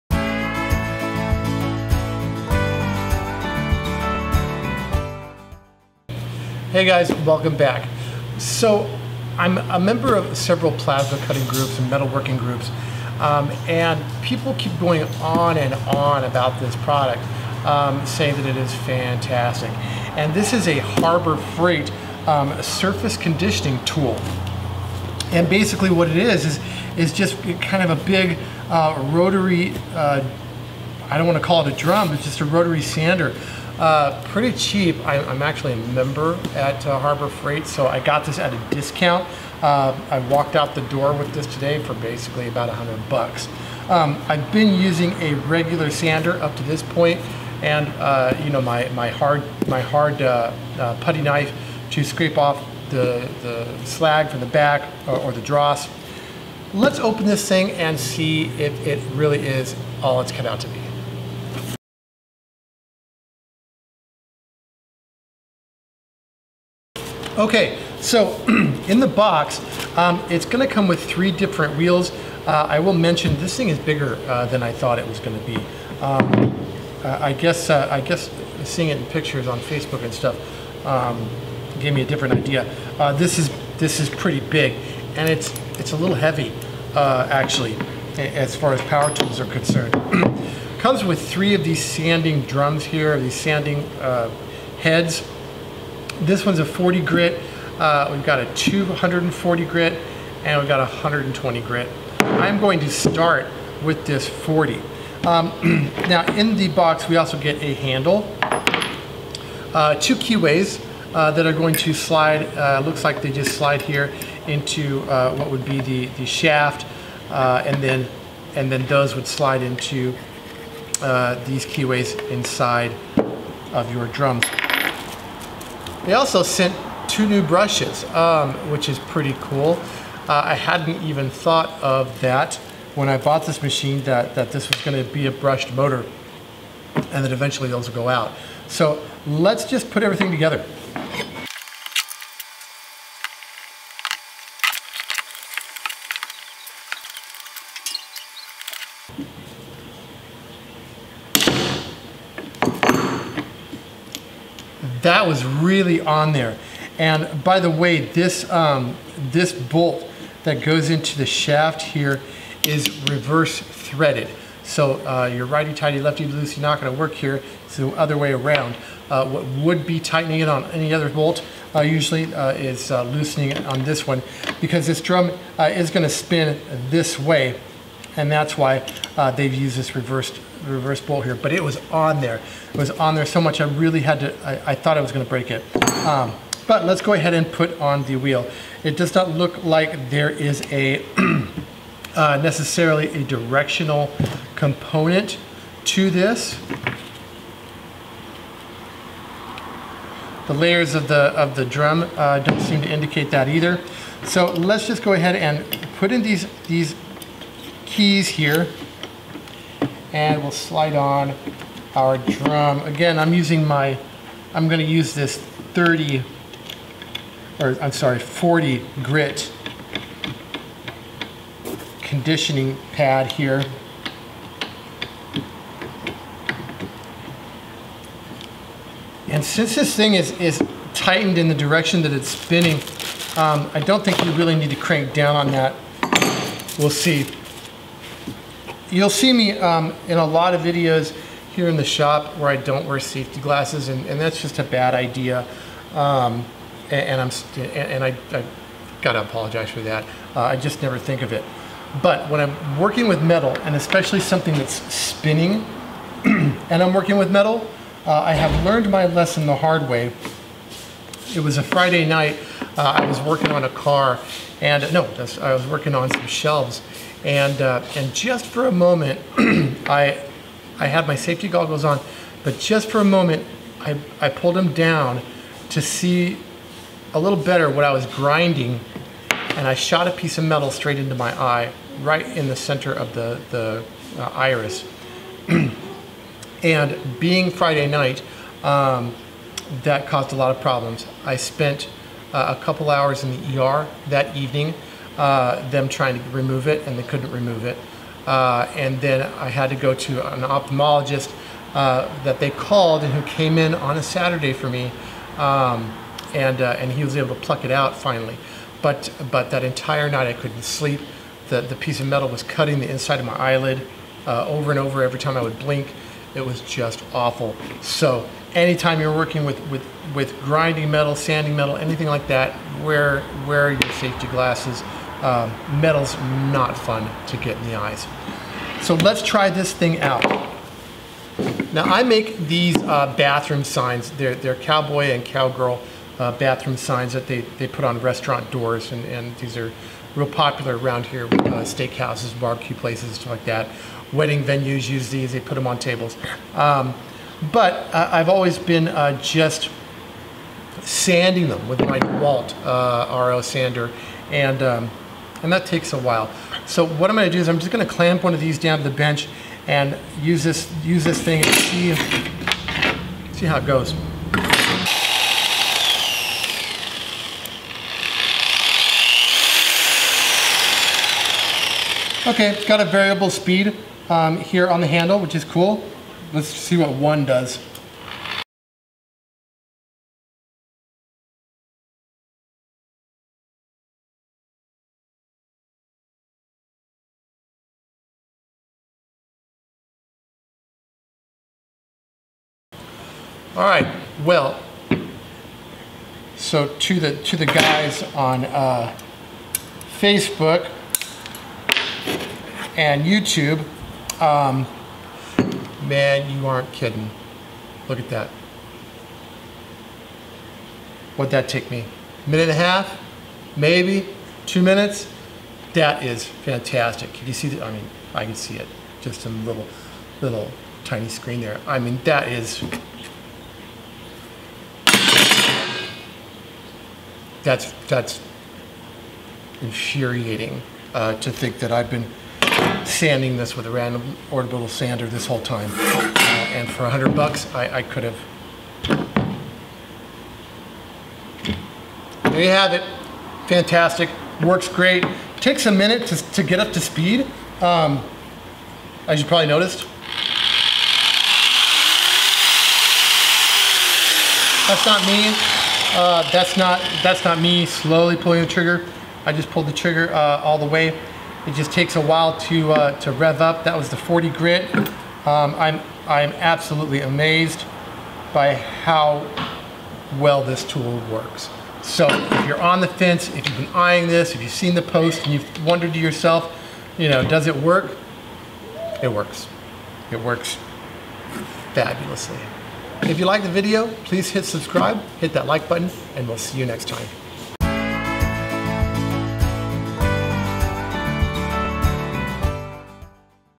Does the Bauer Surface Conditioning tool really work?
There is no shortage of people on the internet that bash on Harbor Freight tools. I myself have picked up some losers there, however, I've also picked up a lot of things that just work perfectly from Harbor Freight and this is one of them.

This Bauer Surface Conditioning tool has been popping up in comments from people in my metal and plasma working groups and I just had to give it a go.

Its available at Harbor Freight of course but it's also available from Amazon for the same in-store price as Harbor Freight.

The BAUER™ 9 Amp Surface Conditioning Tool delivers up to 3700 RPM for removing rust, paint, and conditioning metal surfaces. Built with a front D-handle for maximum comfort and control and lock-on trigger for extended use. Accepts a wide variety of 4 in. x 4 in. sanding drums.

Here's the specs from the Harbor Freight web site.

Variable speed control delivers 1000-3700 RPM for surface preparation applications
High torque 9 Amp motor handles the toughest jobs
All metal gearing for long life and durability
Six speed control dial to match sanding speed to application
Ergonomic inline design reduces fatigue during extended use
Accepts a wide variety of sanding drum accessories
Drum guard keeps debris away from user